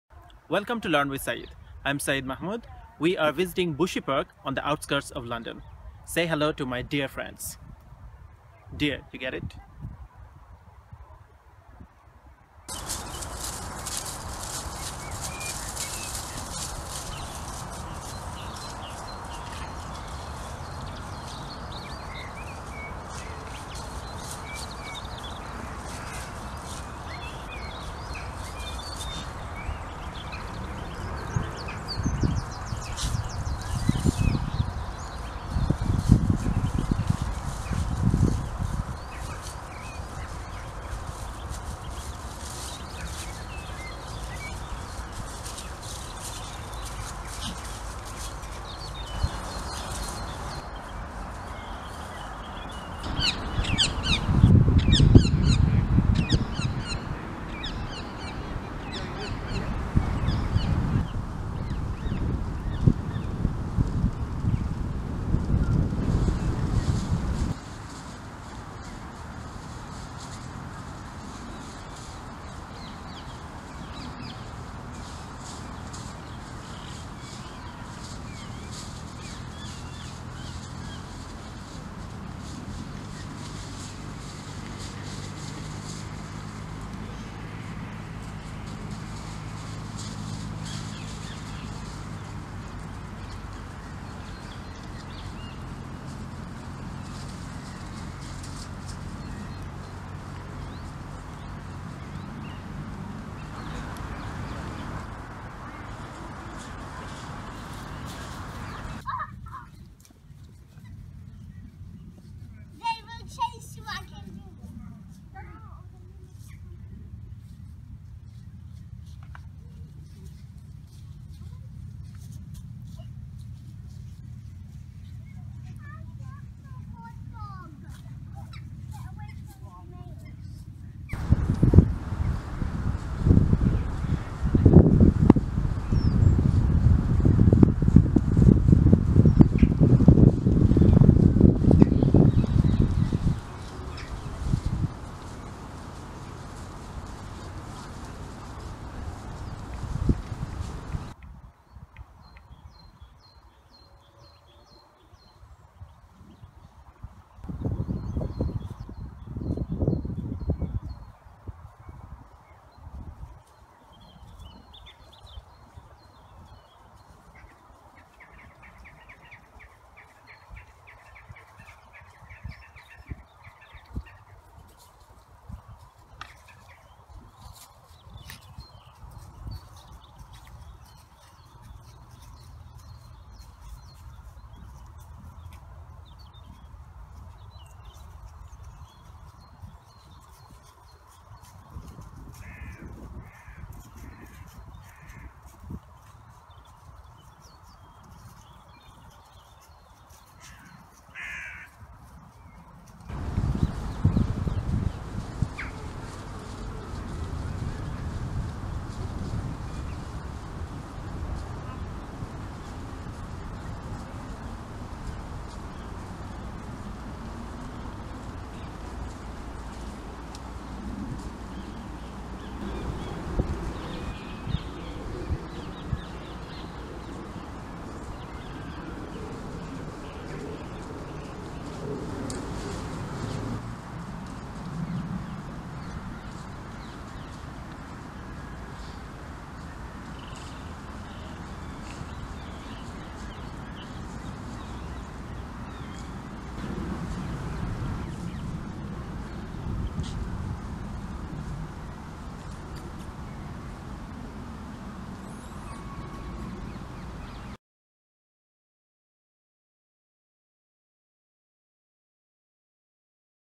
Red and Fallow Deer in Bushy Park London British Wildlife
Every year millions of Londoners and tourists visit Bushy Park, the second largest of the capital's eight Royal Parks.
Bushy Park's mixture of woods, gardens, ponds and grassland makes it a fantastic place to enjoy wildlife. Red and Fallow Deer still roam freely throughout the park, just as they did when King Henry VIII used to hunt here.
There are (as of 2016) currently about 320 deer and their grazing is essential to maintain the high wildlife value of the park's grasslands. Unlike cutting, grazing creates more variation in structure and plant diversity and does not damage the anthills, which add further diversity and character to the grassland.

The herds are kept out of the Woodland Gardens and other protected plantations in order to protect the trees and shrubs there. The Red Deer are the largest mammal native to the British Isles and in the summer their coats are glossy red. Fallow deer, introduced by the Romans, are smaller and their summer coats, usually spotted, vary from a cream to darkish brown colour.

During the autumn the deer 'rut' (breeding season) takes place. The Red stags and Fallow bucks compete for females (known as hinds and does respectively). At this time, the large males roar, bark and clash antlers in a spectacular way in an attempt to fight off rivals and attract as many females as possible. The young are born May - July and are hidden by their mothers amongst the bracken and long grass. The young are very vulnerable at this time and their mothers, being sensitive to disturbance, will defend their babies.